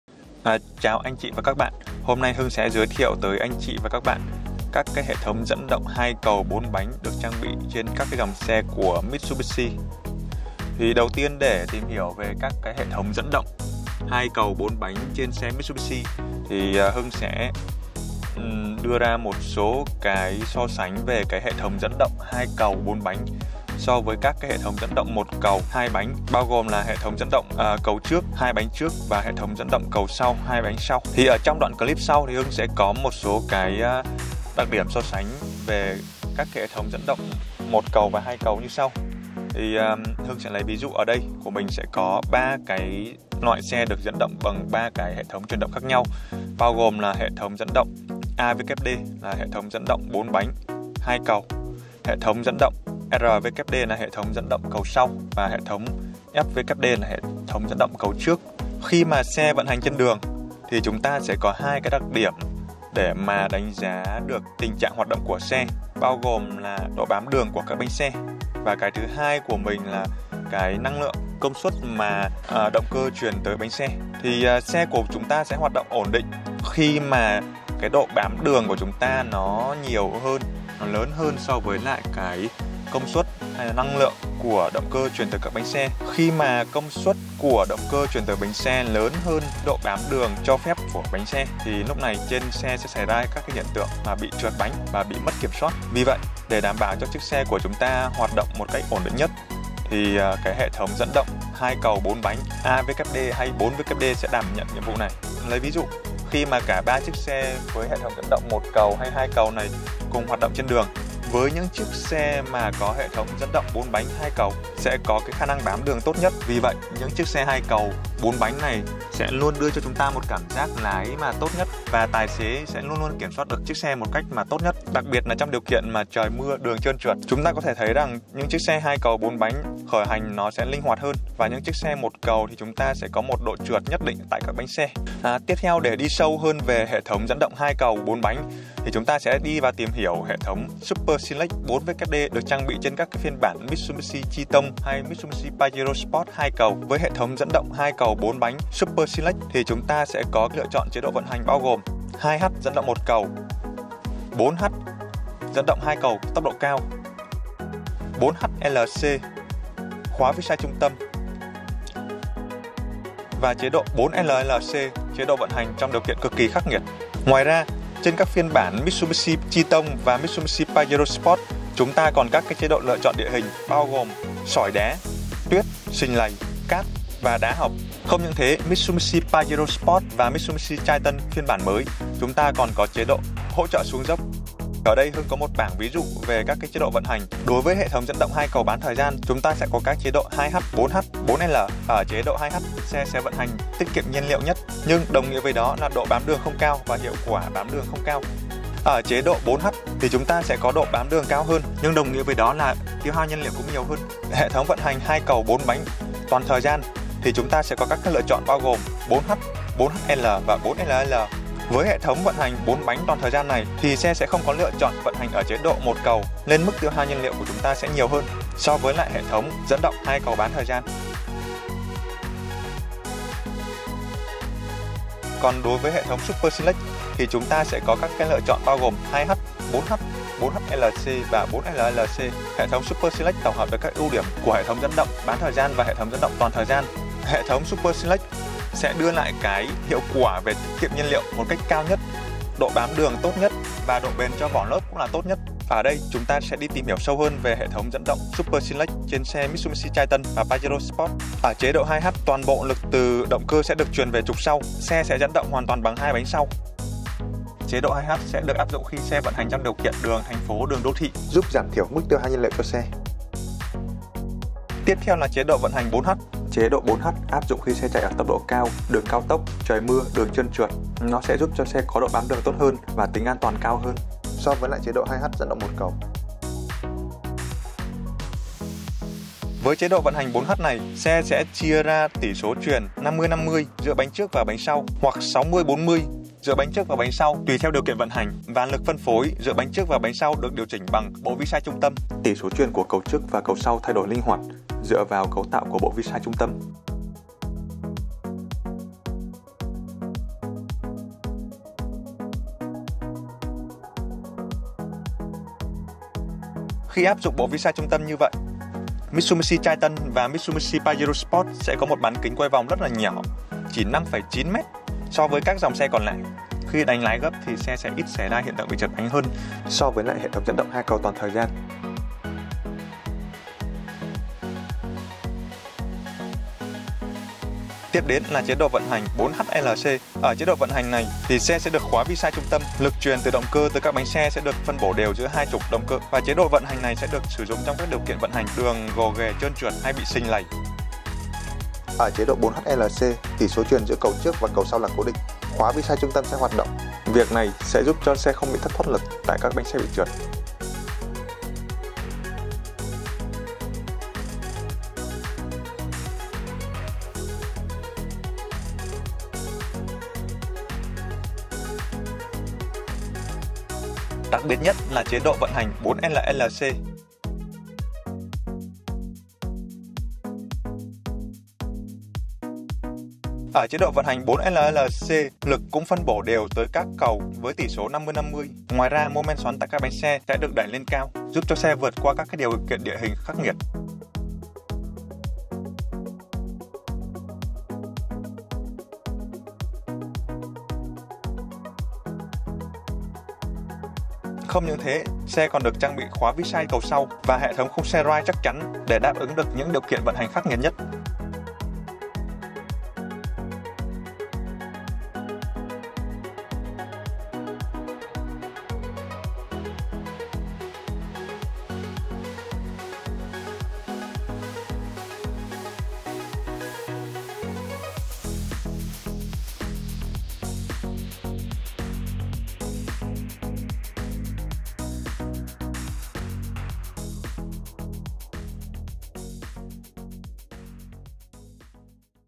4WD-Super Sellect II - Hệ thống 2 cầu đặc biệt của Mitsubishi vận hành thế nào?
Hệ thống dẫn động 4 bánh đặc biệt của Mitsubishi Motors, Super Select 4WD-II mang đến sự thoải mái và tự tin cho người lái khi bạn cần, cho phép chiếc xe thích nghi với từng loại bề mặt. Super Select 4WD-II có 4 chế độ gài cầu để người lái tùy chọn, mỗi cài đặt sẽ phù hợp với điều kiện lái xe riêng biệt.
Chế độ 2H (Dẫn động cầu sau)
Chế độ 4H (Dẫn động 4 bánh toàn thời gian)
Chế độ 4HLc (Dẫn động 2 cầu nhanh toàn thời gian với khóa vi sai trung tâm)
Chế độ 4LLc (Dẫn động 2 cầu chậm toàn thời gian với khóa vi sai trung tâm)